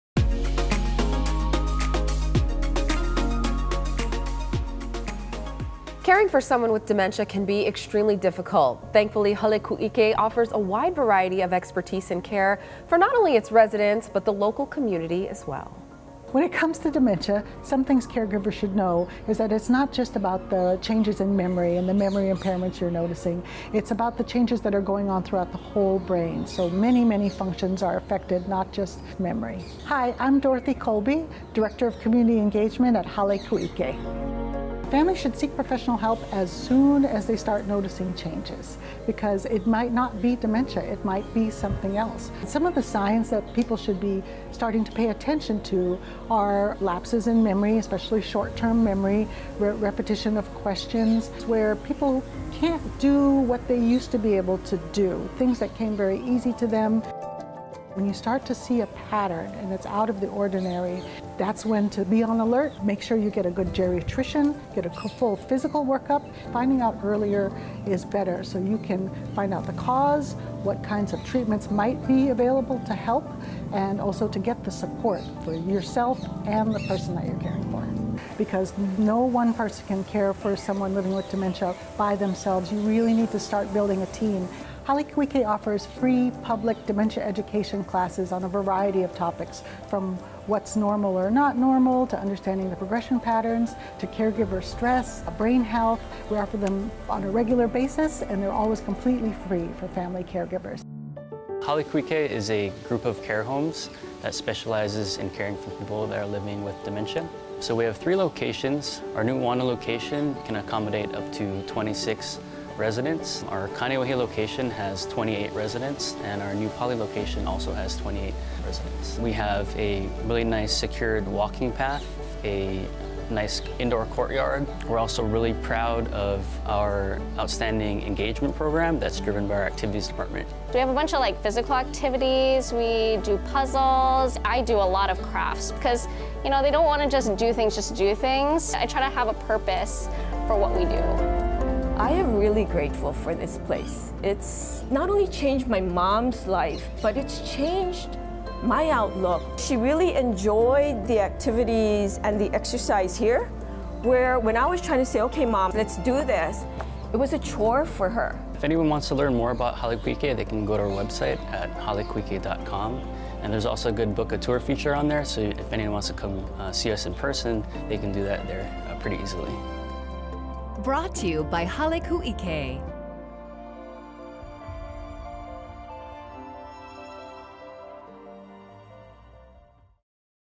Hale Ku'ike Offers a Series of Memory Care Workshops
Hale Ku'ike offers a series of workshops and webinars on dementia care for the community. for more information.